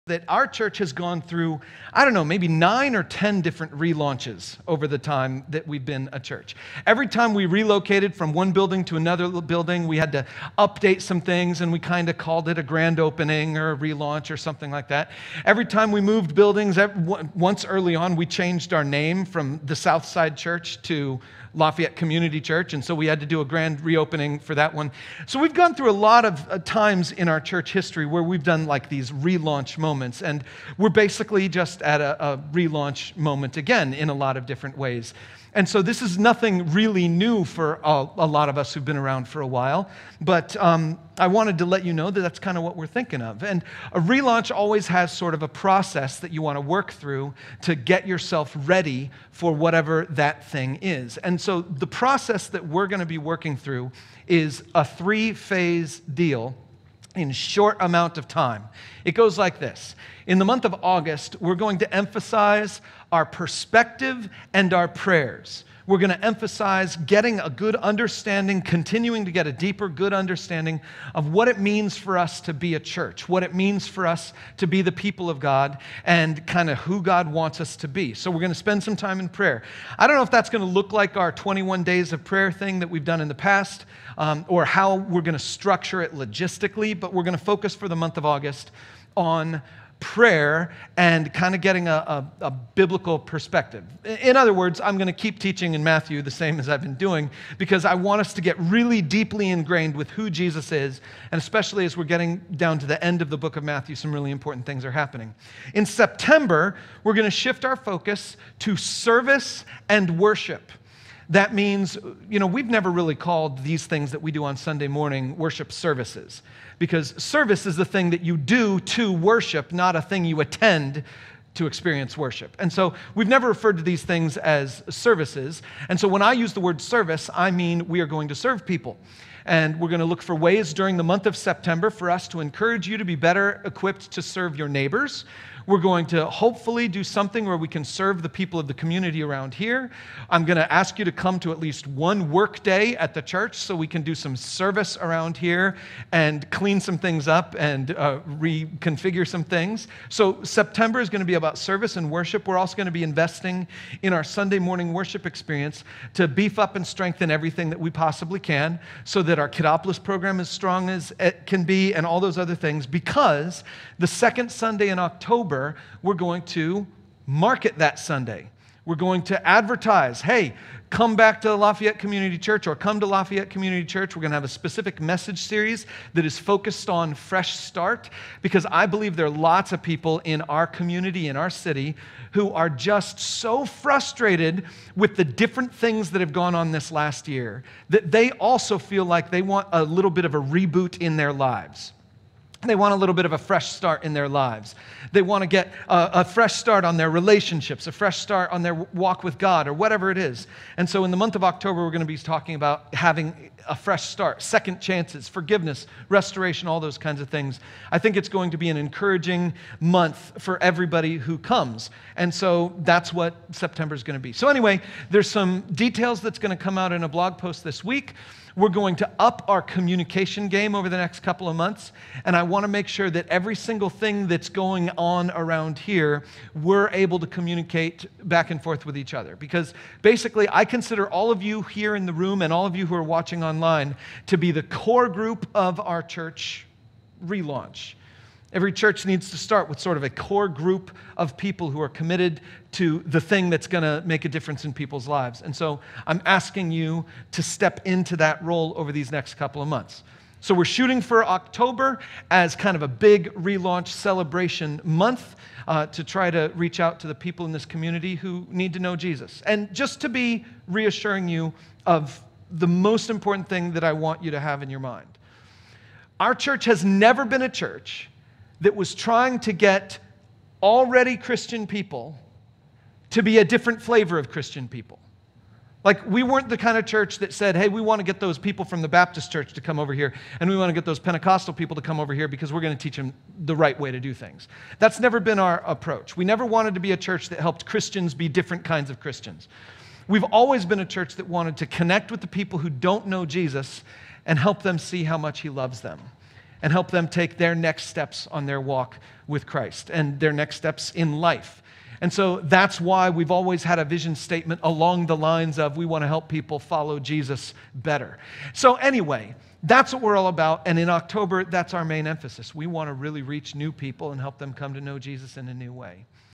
LCC 2021 Fall Relaunch Announcement
On Sunday, July 18, Pastor Jeff gave a brief description of how the next three months are going to look as we prepare for an October church relaunch.